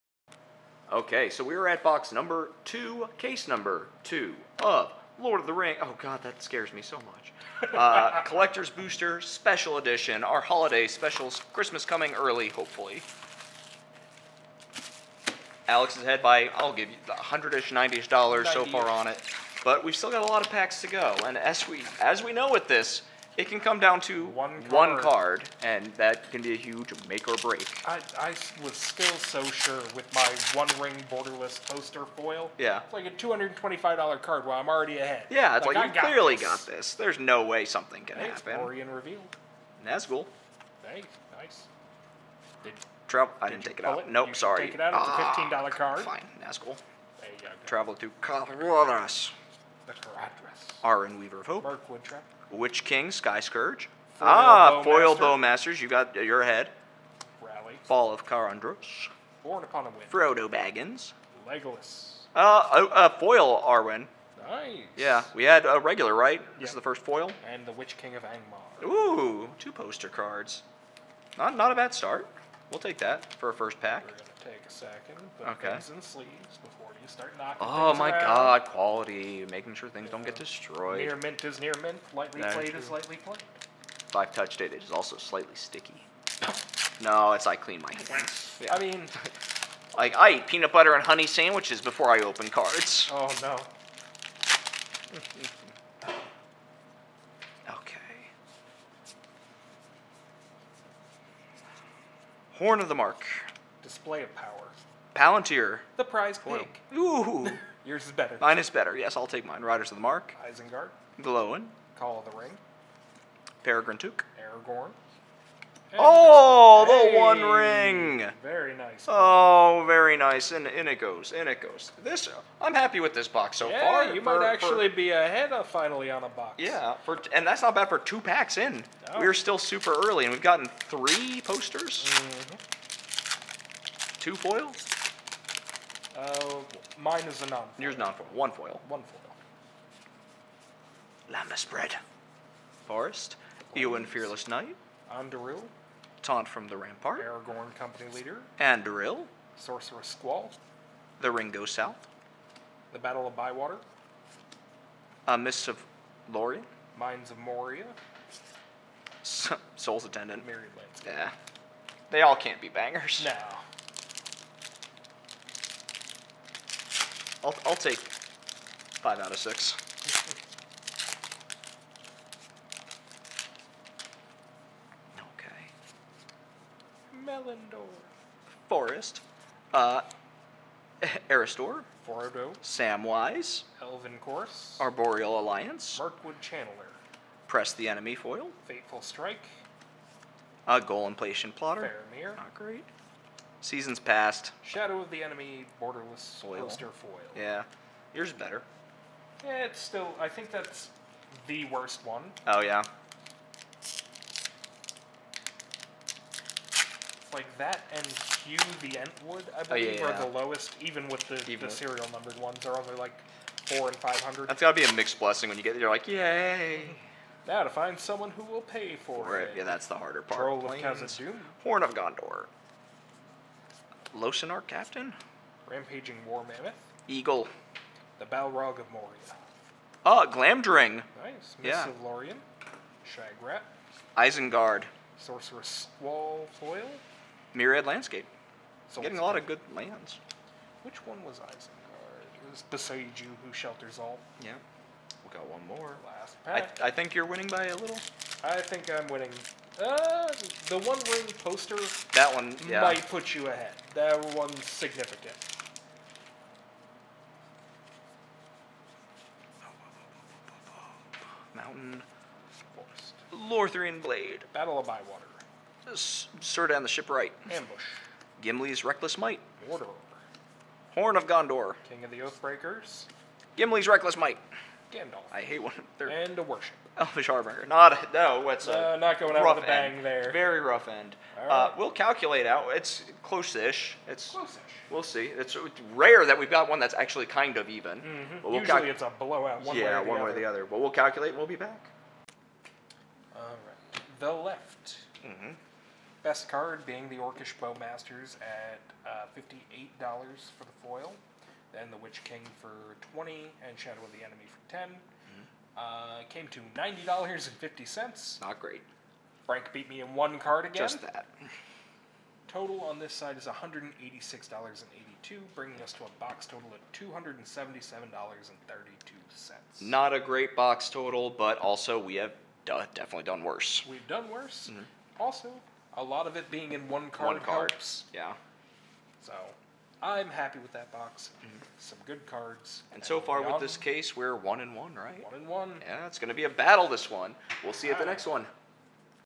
Lord of the Rings: Tales of Middle-earth Special Edition Collector Box (Box 2 Case 2) Box Wars!
Watch Frank and Alex battle it out in box wars!